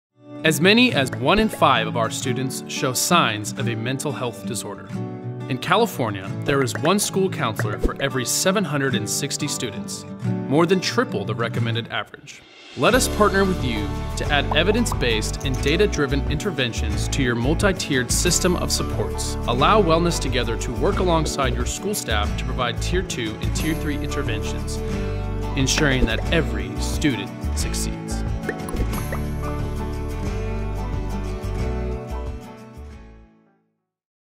Wellness Together: School Mental Health Services
to learn more about Tier 2 & Tier 3 Interventions for your MTSS.

As many as 1 in 5 of our students show signs of a mental health disorder. In California, there is 1 School Counselor for every 760 students, more than triple the recommended average.
Let us partner with you to add evidence-based and data-driven interventions to your Multi-Tiered System of Supports (MTSS).

Allow Wellness Together to work alongside your school staff to provide Tier 2 and Tier 3 interventions, ensuring that every student succeeds.